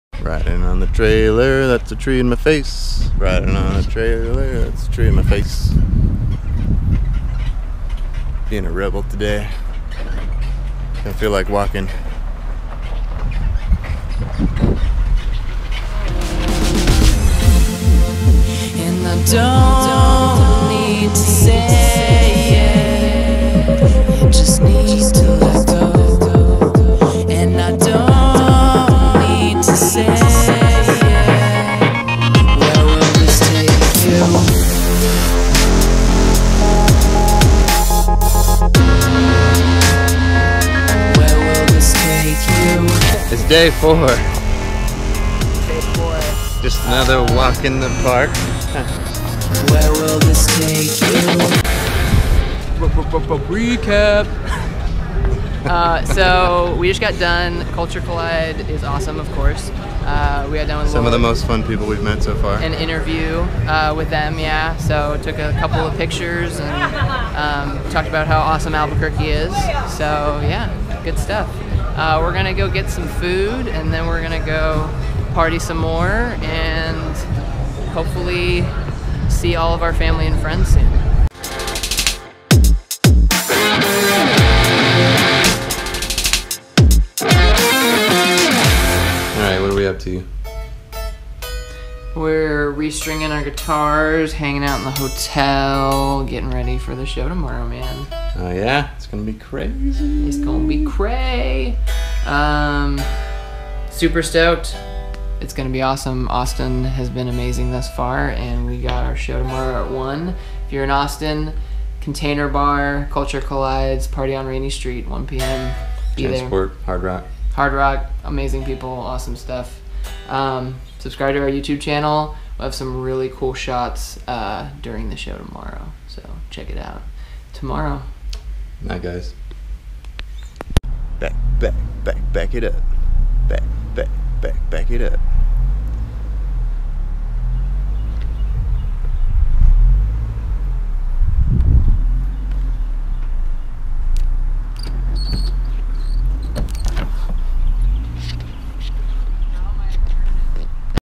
SXSW 2015 - Day 4 [ repel the robot AT SXSW ]
repel the robot travels to Austin for SXSW 2015

Day 4 Recap -

All video shot on GoPro Hero Silver 4